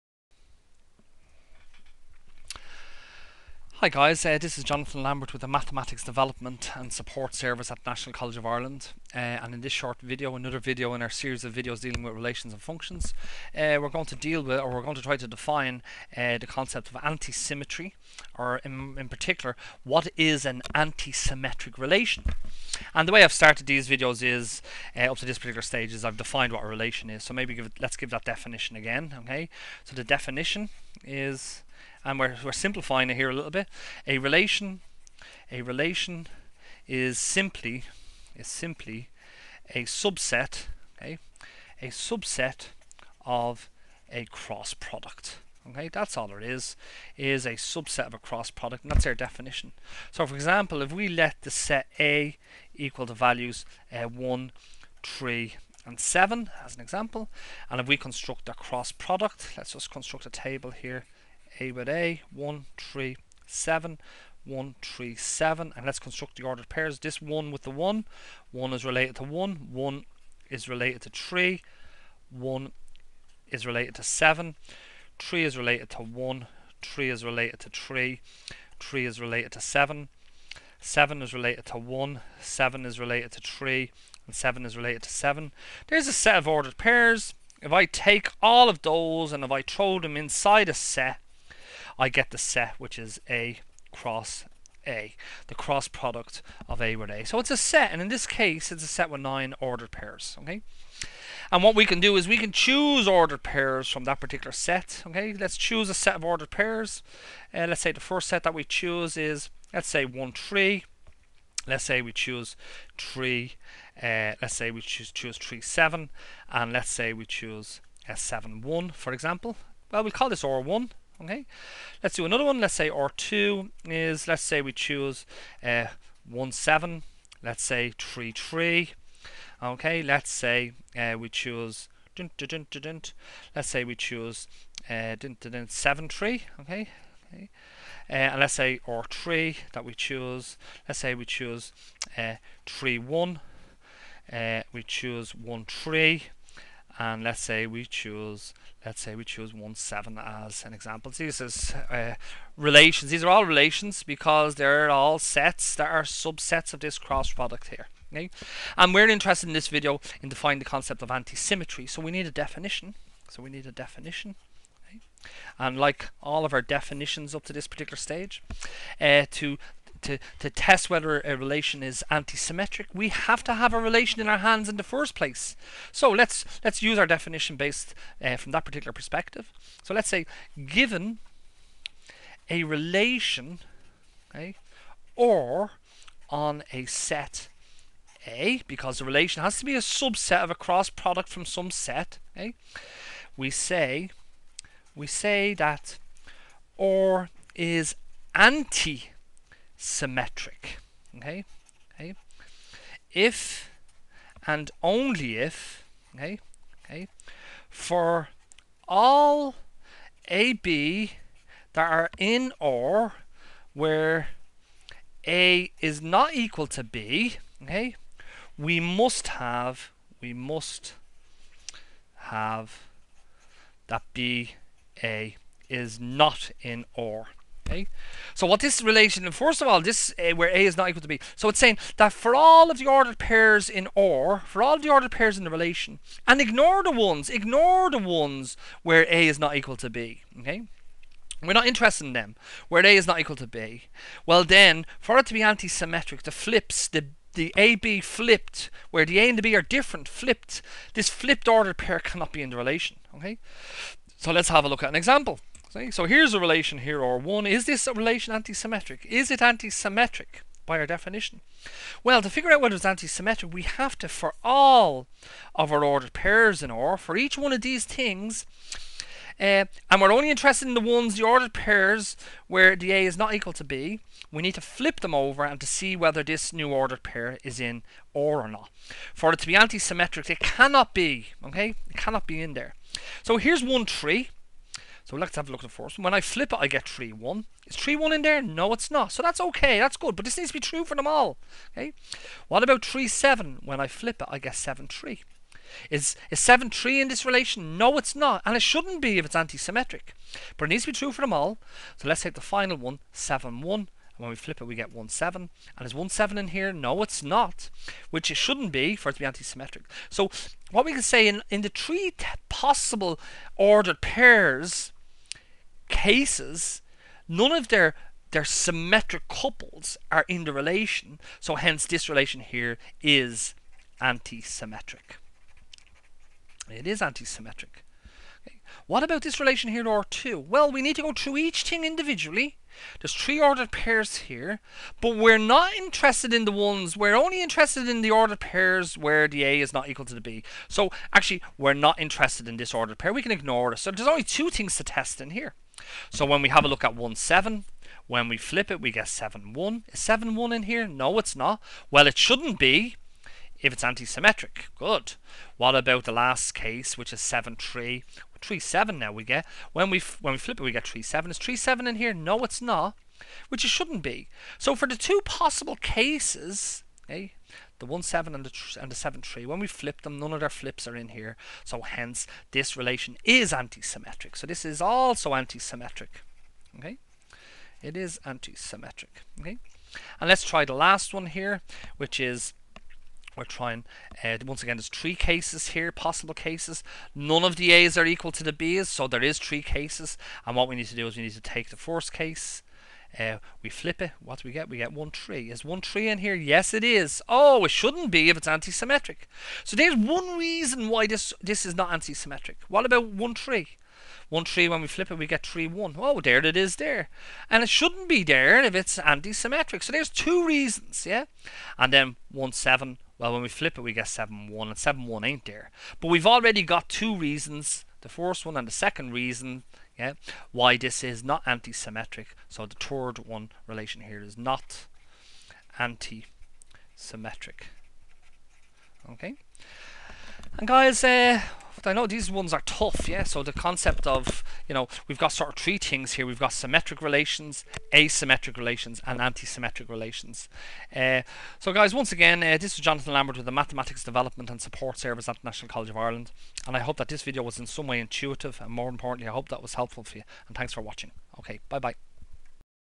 Relations and Functions: What is an Antisymmetric Relation?
In this short video, we define what an Antisymmetric relation is and provide a number of examples.